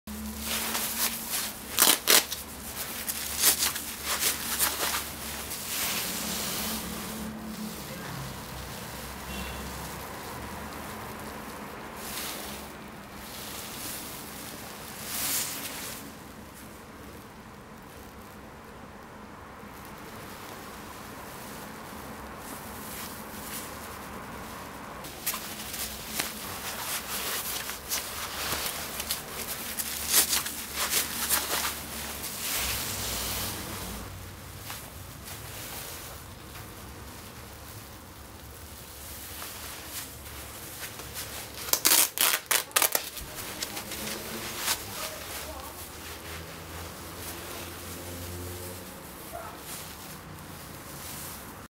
Γάντια μηχανής χειμερινά AGVpro POWERLINERS Motorcycle GLOVES AGVpro
Motorcycle GLOVES AGVpro
Γάντια μηχανής χειμερινά AGVpro POWERLINERS
Με ειδικό κλείσιμο στο καρπό για να φοριούνται & μέσα από το μπουφάν μηχανής
Είναι ελαστικά για καλύτερη εφαρμογή
Ιδανικά για την πόλη